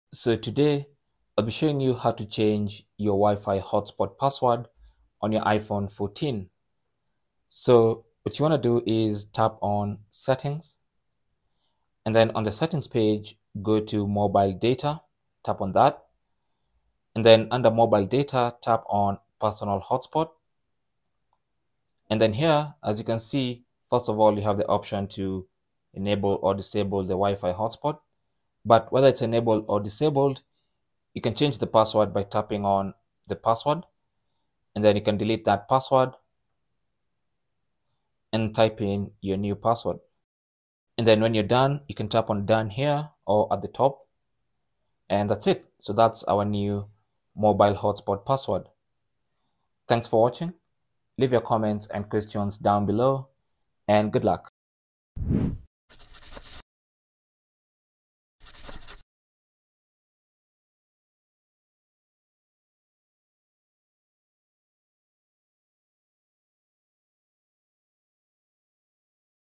How To Change the Hotspot Password On iPhone 14 / iPhone 14 Pro
Here's how to change the hotspot password of iPhone 14.
By default, you're provided a hotspot password on your iPhone 14. If you find it hard to remember your hotspot password, you can simply change it.

How To Change HotSpot Password
Go to Settings
Tap on Mobile Data
Tap on Personal Hotspot
Tap on WIFI Password to change the password
Tap on Done to save the changes

Cell Phone Tripod Adapter